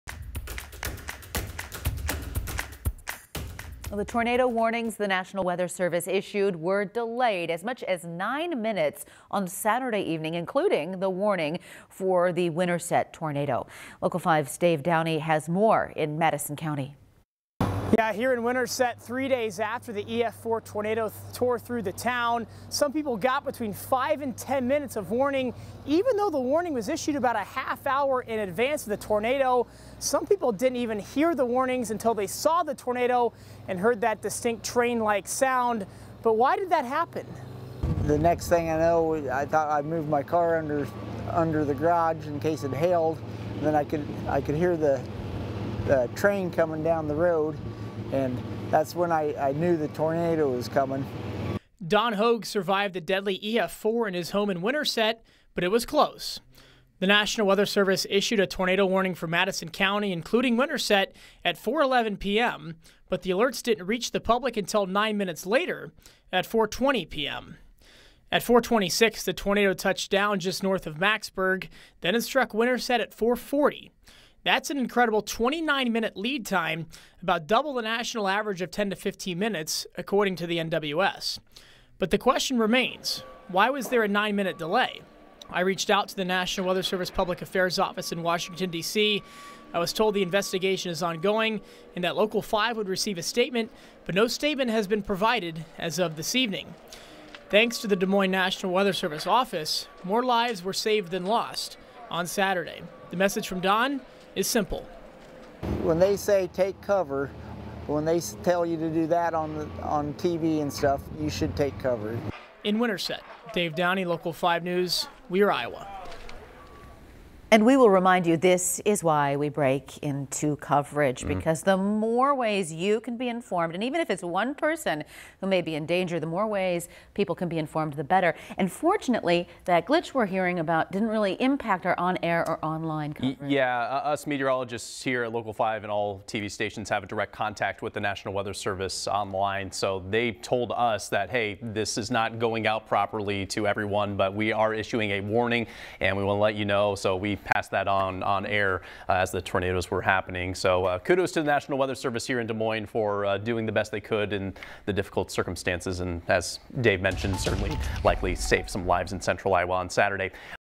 Winterset, Iowa tornado: Why was warning alert delayed?
The EF-4 tornado that struck Winterset on Saturday evening peaked with winds of 170 mph, and took six lives in Madison County.

But there was a problem with the tornado warnings being issued by the National Weather Service in Des Moines, and it wasn't the local bureau's fault.